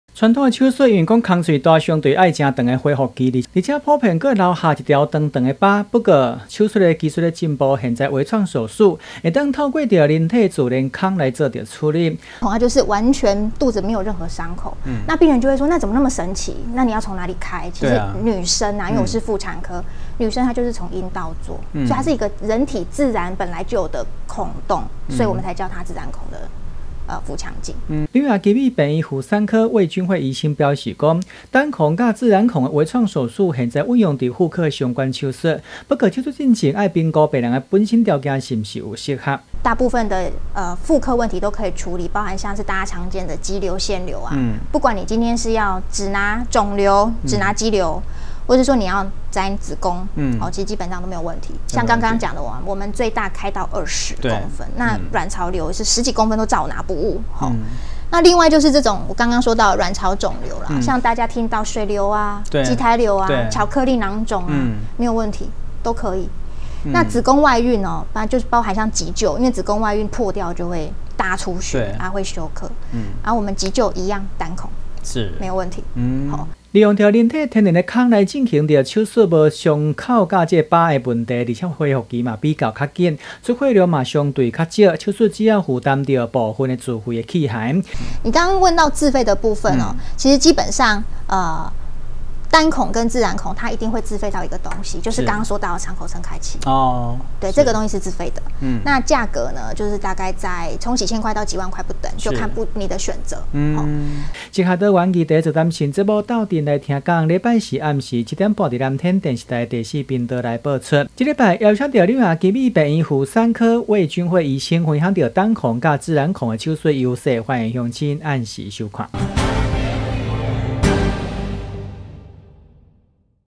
233 手術選擇日新月異 逗陣分享自然孔微創術20200227
手術選擇日新月異 逗陣分享自然孔微創術
隨著醫學技術的進步，現在手術選擇越來越多，除了傳統手術之外，現在還有單孔以及自然孔微創手術，透過人體自然孔手術，看不出傷口之外，恢復時間也縮短很多，這禮拜四所播出的逗陣來聽講節目，邀請柳營奇美醫院婦產科魏君卉醫師，分享最新的單孔及自然孔微創手術優勢，歡迎鄉親準時收看。